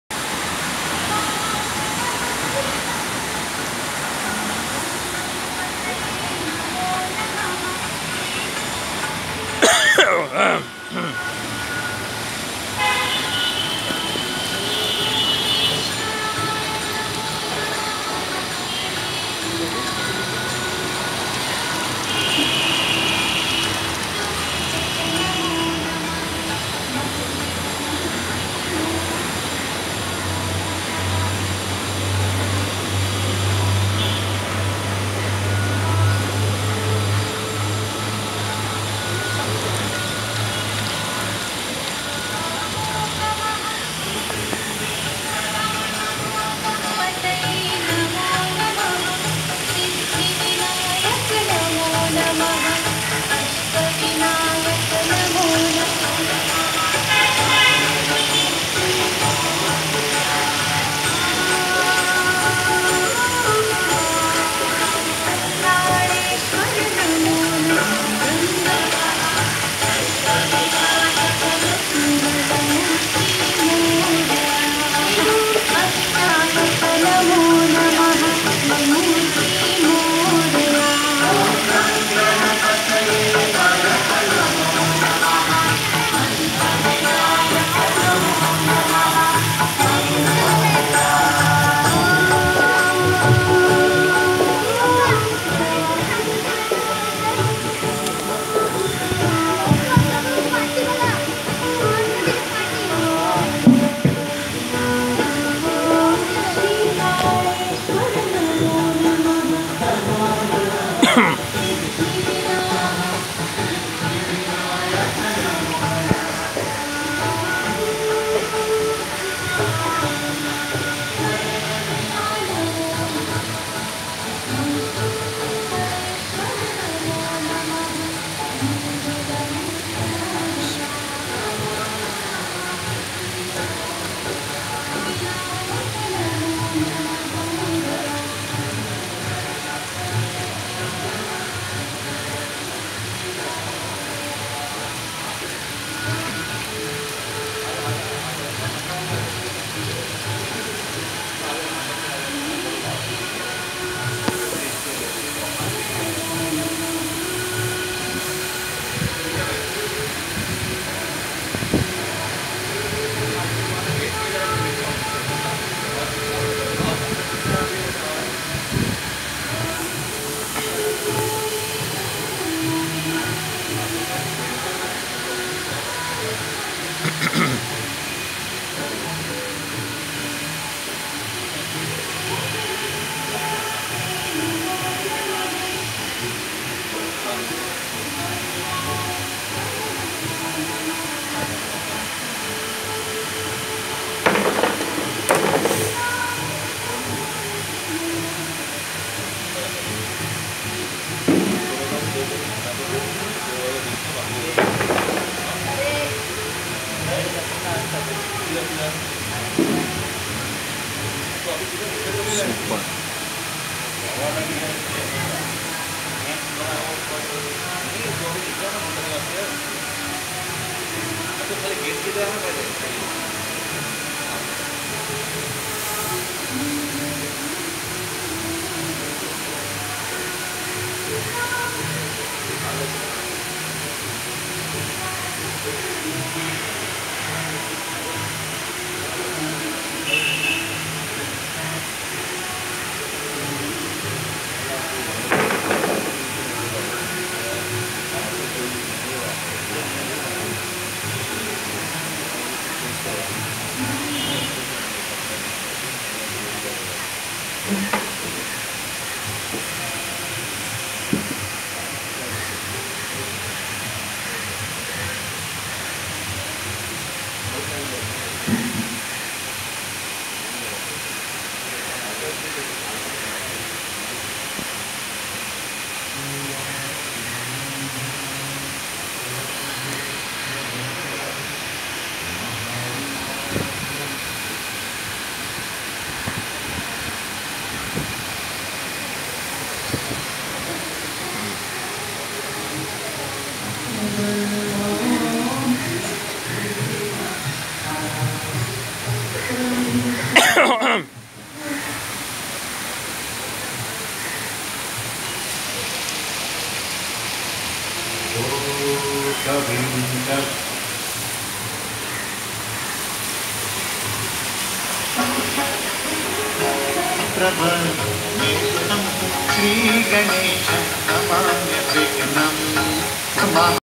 Ganesh Utsav 2018 Matunga: Waters of India wonders of Tejukaya Park
A completely attractive decoration for this pandal featuring different kinds of fountains and waterfalls right from outside to inside. Hats Off to the crew.